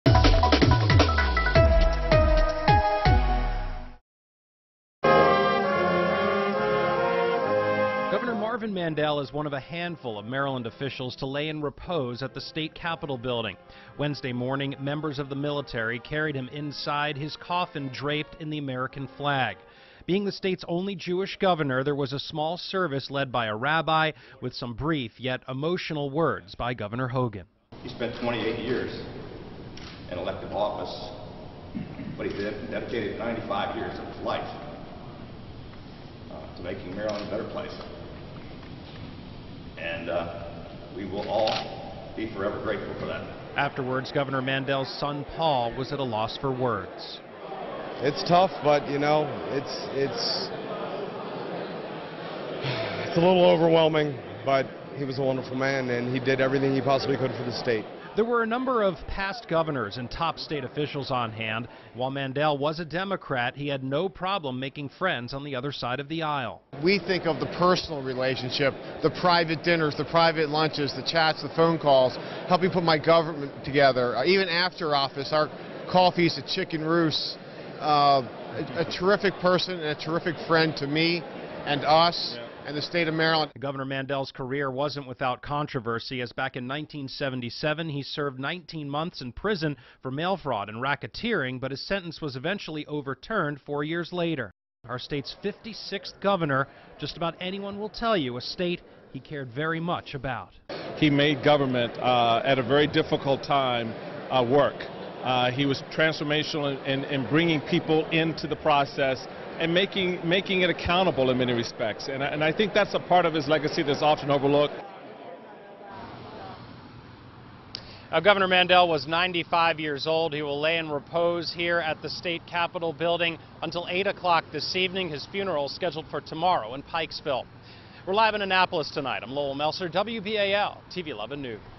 Gov. Marvin Mandel lies in repose in State House
Top state officials past and present joined friends and family Wednesday in Annapolis to pay respect to former Gov. Marvin Mandel, who died Sunday at the age of 95.